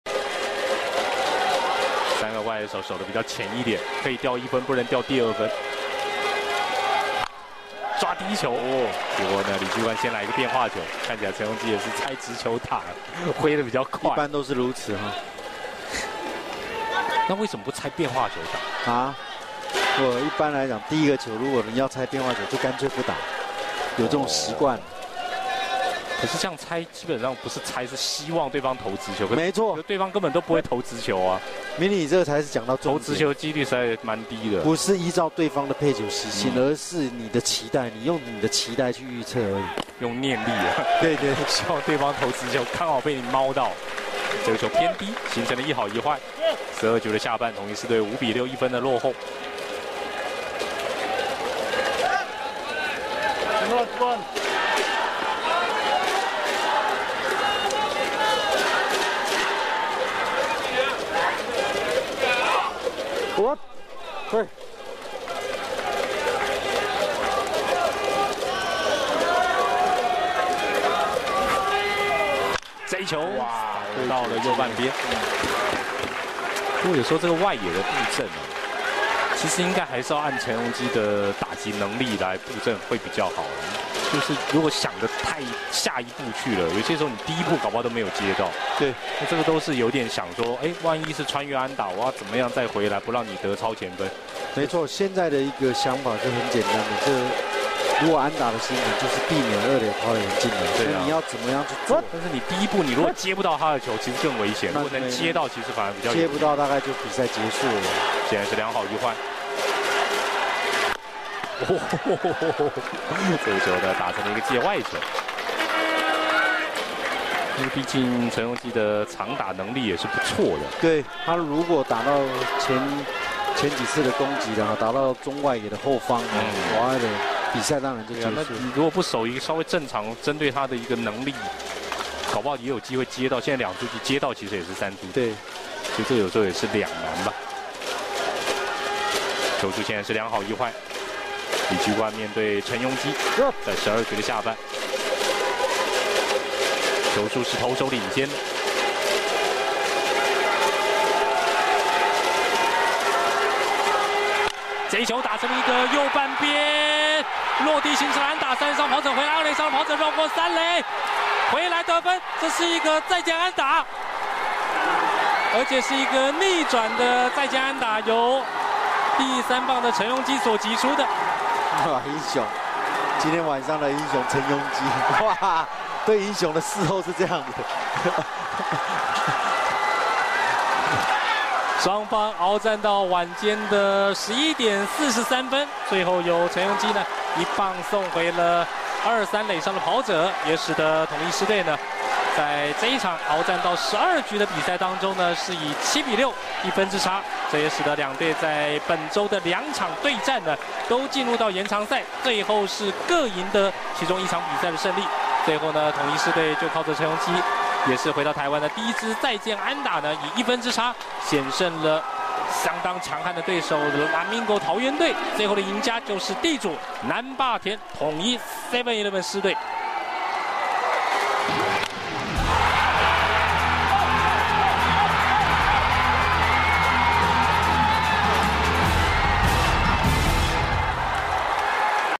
20110421 陳鏞基再見安打
陳鏞基再見安打 苦主李居冠 最後一球145KM